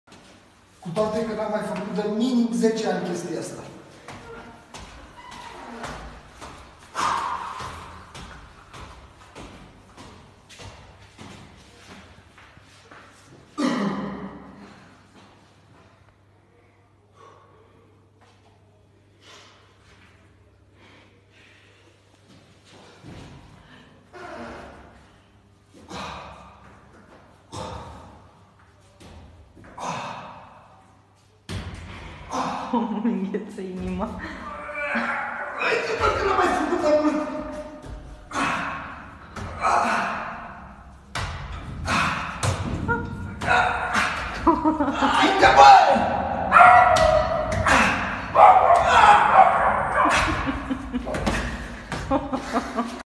Marian Dragulescu te provoc
Provocare. . Marian dragulescu
Provocare. . Marian dragulescu
Știu ca ai cucerit lume olimpica.mondiala .Și european.  De  mim 5 ori în parte
Dacă vei acepta.. Mă voi antrena. O luna
Și martori mie dmz din cer ca  cobor  de la etajul 5
Și dacă vei fi accepta  și cu a 2 provocare.jur pa famila mea daca nu cobor de la 10 după 2 luni de antrenament.

Pentru prietwni mei câțiva dintre iei nu am mai făcut ăsta din 2009 nici măcar cel puțin sa fac un antrenament de încălzire .dasta măm și balansat umpik.si numai vorbim 92de kg😂 .Va mulțumesc și aștept provocare.iar voi dacă vreți sa o vedeți. . Și ca mă țin de cuvânt.  Dați distribuirii.  Sa ajungă la  olimpic nostru sa vedem care se tine de cuvânt. ..  Va pup și va doresc multa santate toți 😋😋😙😍😍😍😍